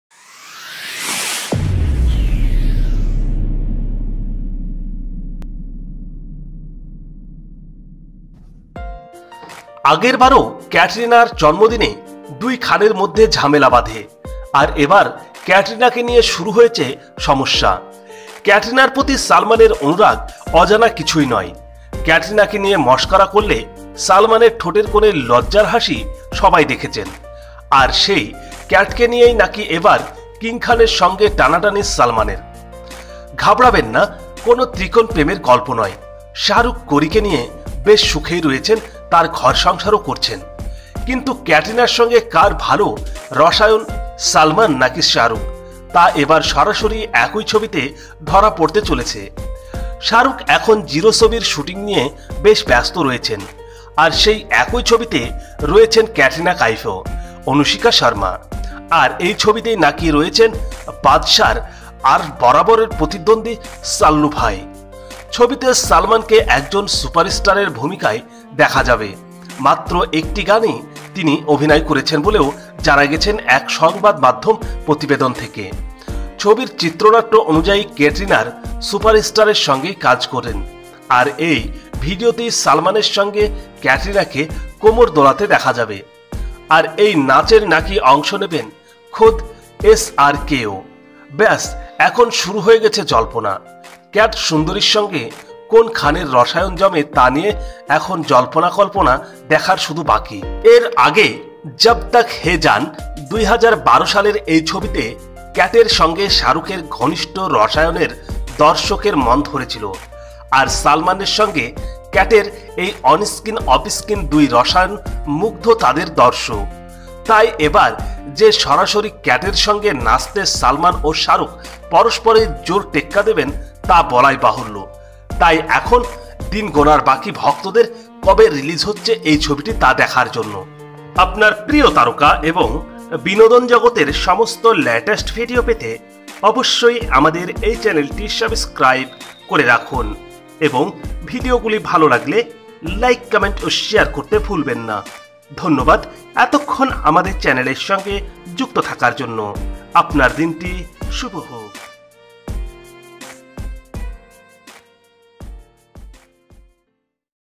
ক্যাটরিনাকে নিয়ে ‘টানাটানি’ সলমন-শাহরুখের! আবার রেষারেষি দুই খানের মধ্যে !! Salman, Katrina
ক্যাটরিনাকে নিয়ে ‘টানাটানি’ সলমন-শাহরুখের! আবার রেষারেষি দুই খানের মধ্যে  !!  Salman Khan Katrina Kaif

ক্যাটরিনার প্রতি সলমনের অনুরাগ অজানা কিছু নয়। ক্যাটরিনাকে নিয়ে মশকরা করলে সলমনের ঠোঁটের কোণে লজ্জার হাসি সবাই দেখেছেন। আর সেই ক্যাটকে নিয়েই নাকি এবার কিং খানের সঙ্গে টানাটানি সলমনের!

►Source   :
►photos   :     facebook page profile
►Videos  :